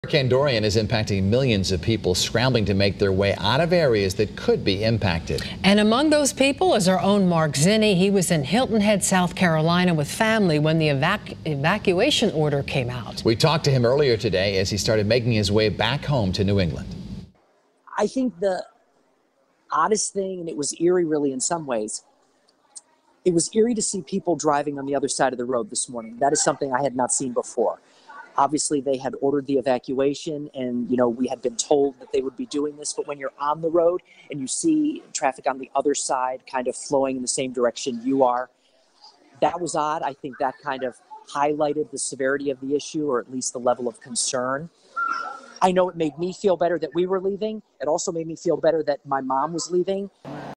Channel 3's Mark Zinni evacuates from South Carolina before Dorian
Channel 3 anchor Mark Zinni had to evacuate from Hilton Head, South Carolina due to Hurricane Dorian.